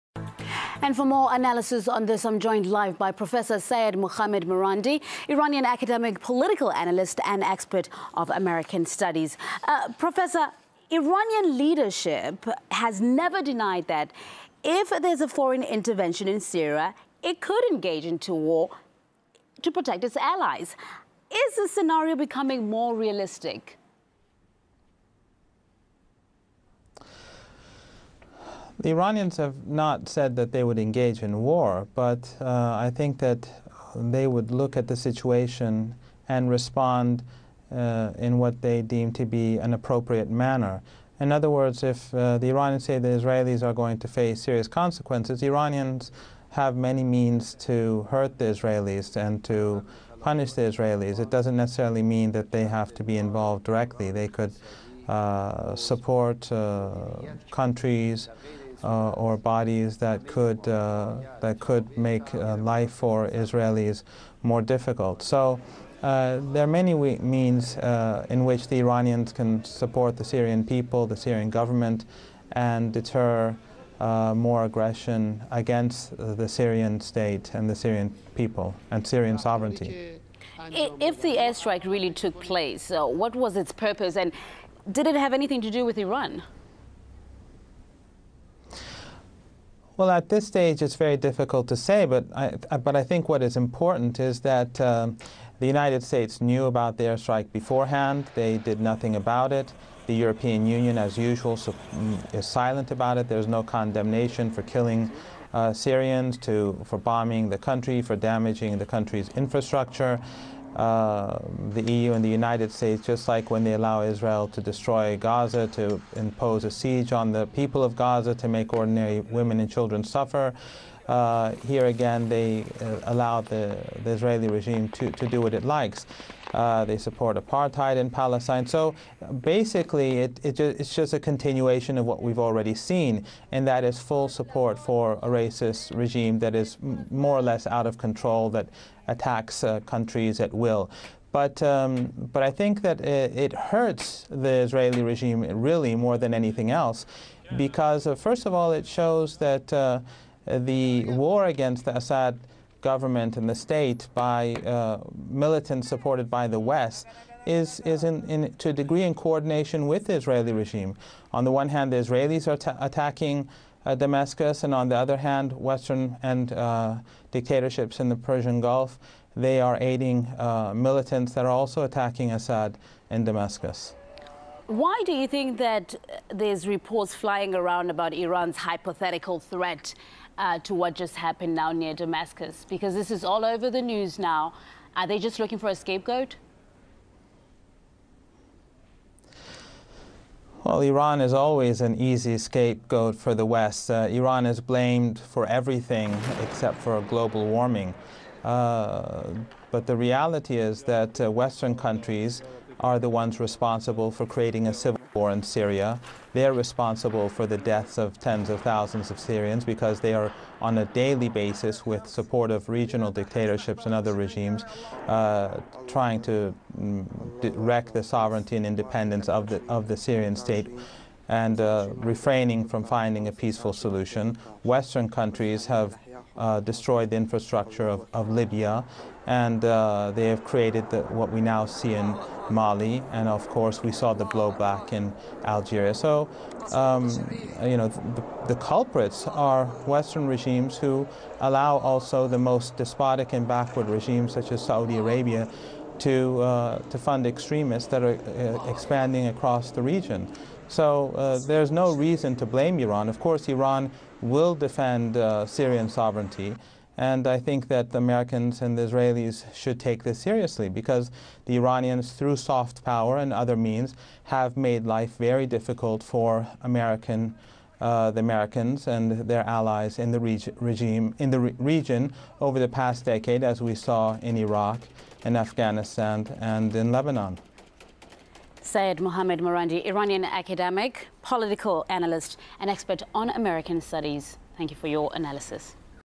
WW3 in ACTION Syria THREATENS RETALIATION for Israeli AIRSTIKE Iran WILL defend Syria
Syria has filed an official complaint to the United Nations over an Israeli airstrike on a military research center near Damascus. The move followed a threat from Iran that Israel would face "serious consequences" for the attack. RT spoke to Prof. Seyed Mohammad Marandi - Iranian academic and political analyst. He says that Israel should be concerned about Iran's possible answer to the airstrike in Syria.

Iranian Foreign Ministry Spokesman Ramin Mehmanparast has condemned Israel's airstrike on Syria, saying that the Tel Aviv regime seeks to create insecurity and instability in the region. "The operation conducted by the aggressive Zionist forces against some targets in Syria last night shows that this regime is after creating instability and insecurity in regional countries as well as weakening the resistance movement," Mehmanparast said in an interview with Press TV on Thursday. He went on to say that Israel's move also indicates that the Tel Aviv regime continues its crimes and aggressions whenever it finds the opportunity.

The Iranian Foreign Ministry spokesman warned that those who are creating tension, insecurity and instability in Syria are "in fact voluntarily or involuntarily laying the ground for more violations and aggressions by the Zionist regime [of Israel]." "Regional countries should be aware of the main danger in the region. They [West and Israel] are trying to create a rift between Islamic countries in the region. They are also trying to create unrest in countries that are in the frontline of the resistance movement against the occupying Zionist regime [of Israel]," he explained
Category
News & Politics
License